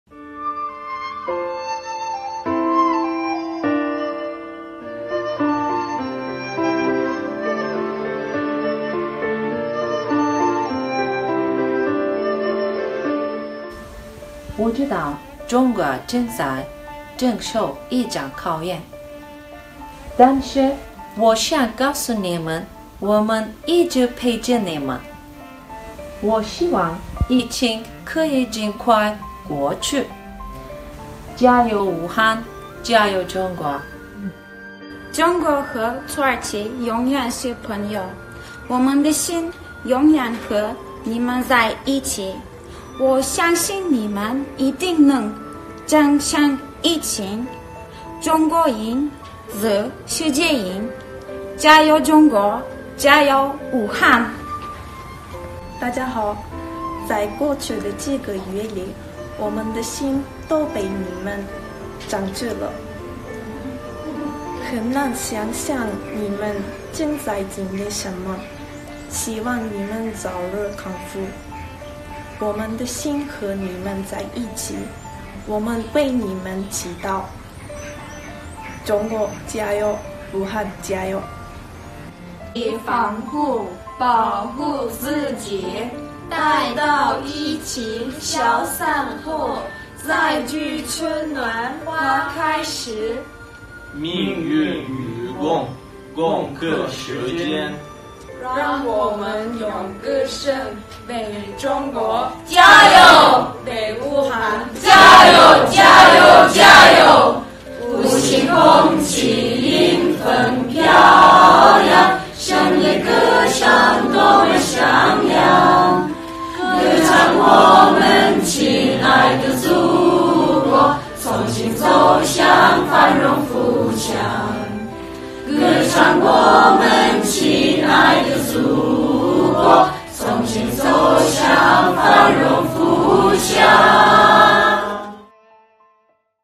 Okan Üniversitesi Çince bölümünde okuyan Türk öğrencilerden Çin’e destek mesajı
🗺️ CRI TÜRK Haber Merkezi Tarafından Hazırlanan,

Çin ve Türkiye Gündemine Dair Son Gelişmeler.

Çin ve Türkiye'den Haberdar Olmak için